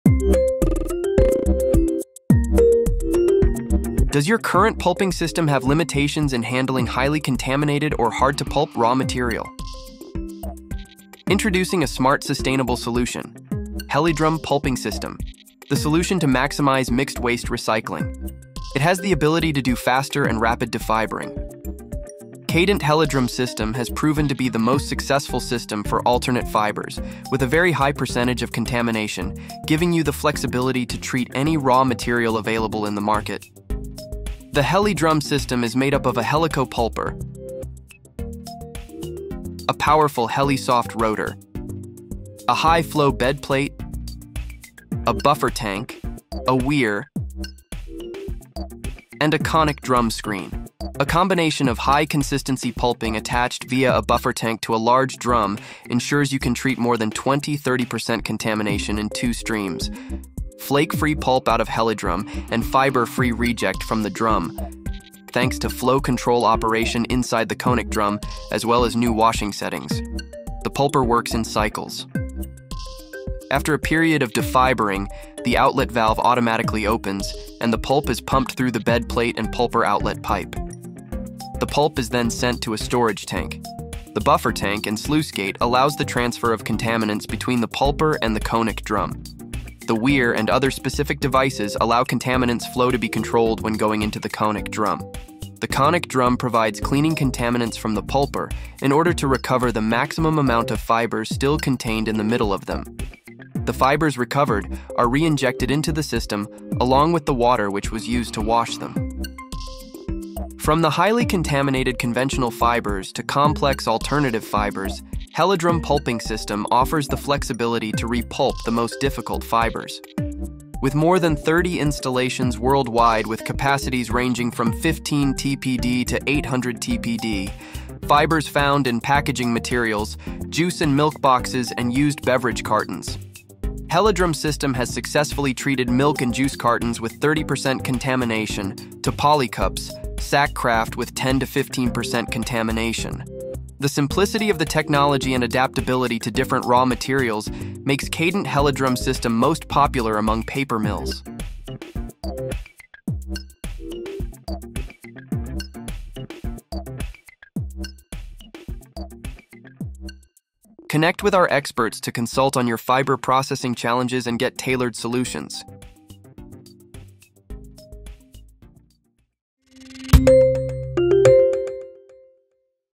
Helidrum Pulping System
Meet the future of pulping: the Helidrum Pulping System by Kadant. Engineered for maximum mixed-waste recycling efficiency, Helidrum excels in the rapid defibering of even the most challenging raw materials. Its cutting-edge design combines a Helico Pulper, Heli-soft rotor, high flow Bed Plate, Buffer Tank, Weir, and Konic Drum Screen to tackle over 20-30% contamination with ease, delivering flake-free pulp and fiber-free rejects.

This advanced system not only streamlines the pulping process through its effective flow control and innovative washing settings but also ensures the recovery and reintegration of fibers, maximizing resource use.